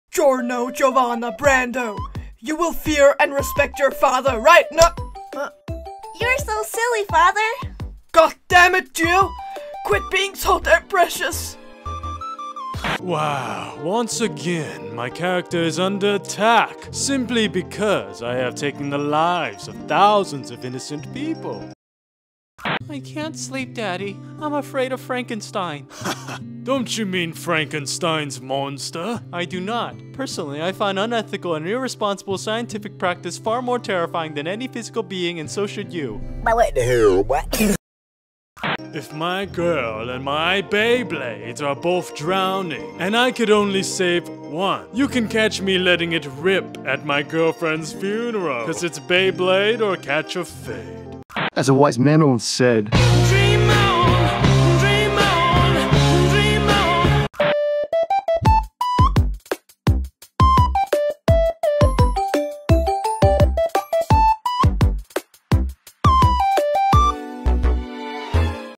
If Dio Met Baby Giorno (JoJo Comic Dub)
What if Dio met baby Giorno? :3 Find out in this adorable and bizarre mini jojo comic dub compilation! Credits and Permission

and @JolyneMeme

What if Dio and Giorno met? What if Dio met kid Giorno?!
In these JoJo comic dub Giorno and Dio share wholesome and educative moments with each other XD

As always, hope you enjoy and have a delightfully Dio-tastic day~